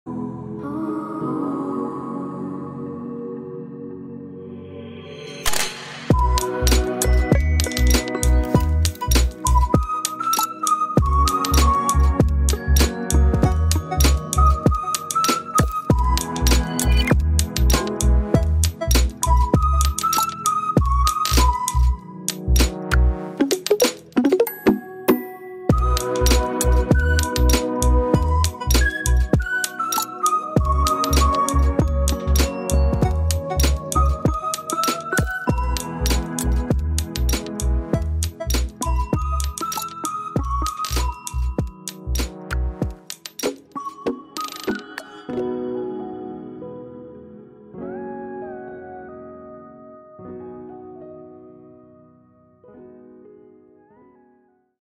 Stitching culottes for my kimono outfit
Putting my old pattern assignments to use. Stitched these culottes for my kimono

I used my embroidery patch on the culottes.

Lemmi know in the comments if you have any suggestions for the next video